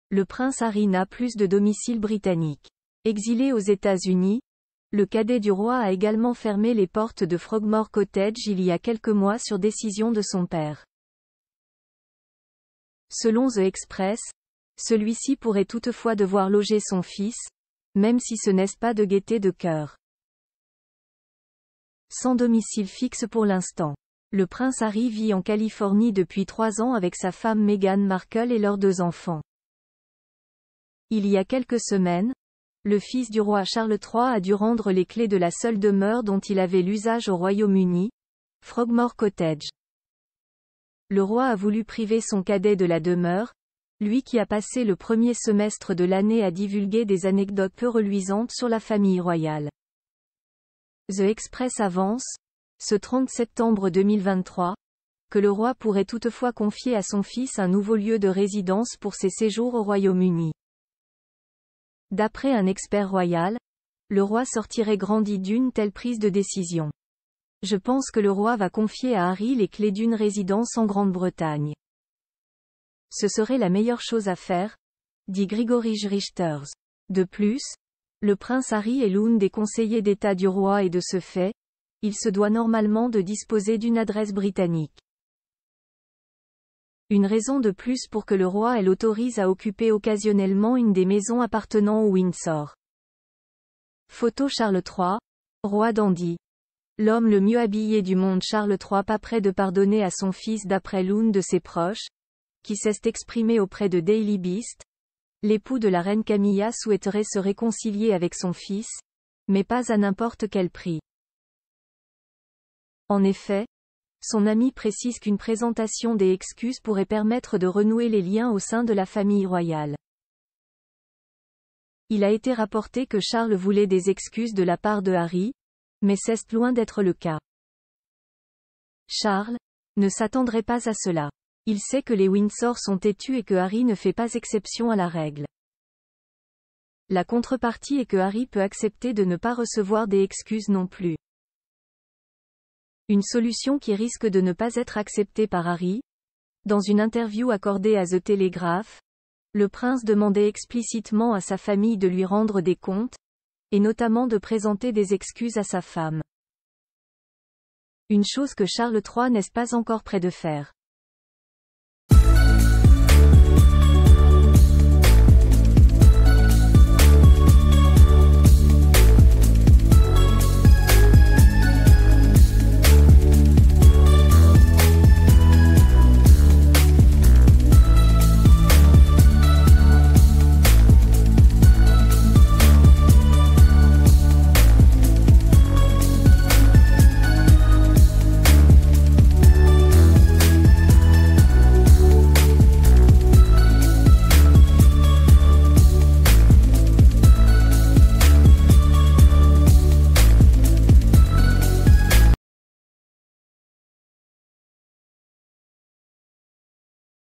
Charles III : le roi prêt à accorder un pied à terre à son fils Harry malgré la brouille ?
Le prince Harry n'a plus de domicile britannique. Exilé aux ÉtatsUnis, le cadet du roi a également fermé les portes de Frogmore Cottage il y a quelques mois sur décision de son père. Selon The Express, celuici pourrait toutefois devoir loger son fils, même si ce n'est pas de gaité de coeur.   Sans domicile fixe pour l'instant. Le prince Harry vit en Californie depuis trois ans avec sa femme Meghan Markle et leurs deux enfants. Il y a quelques semaines, le fils du roi Charles III a dû rendre les...